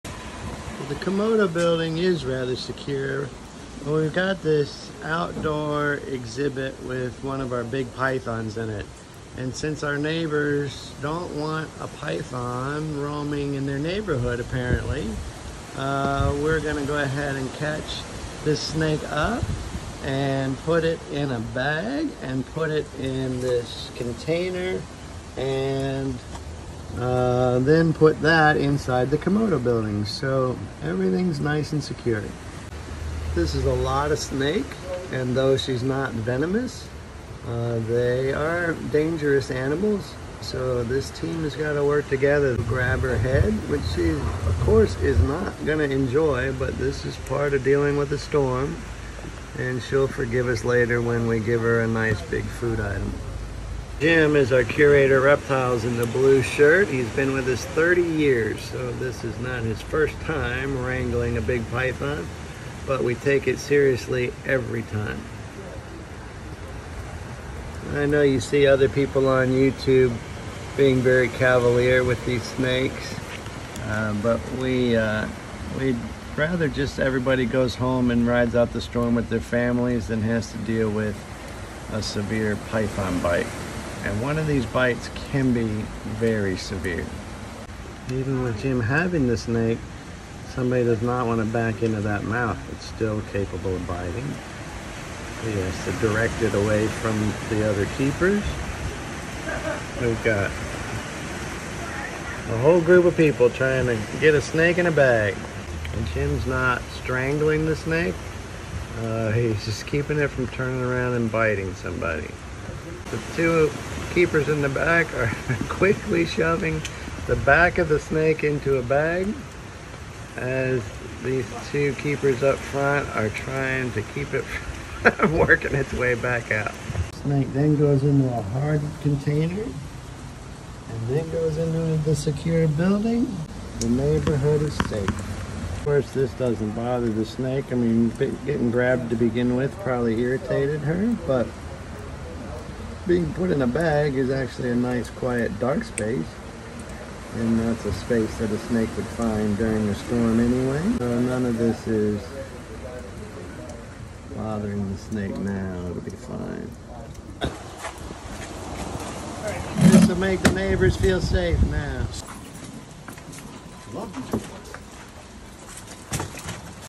Securing Snakes for the Hurricane
Enjoy the calm and tranquil narration of how we secure the giant Reticulated Python at the Zoo, by our Zoo Director.

As per hurricane protocols, we secure our giant snakes and venomous snakes in breathable bags + locked boxes + an inside our hurricane-safe reptile building.

This video is from 2022, but we follow the same procedures in every Hurricane event.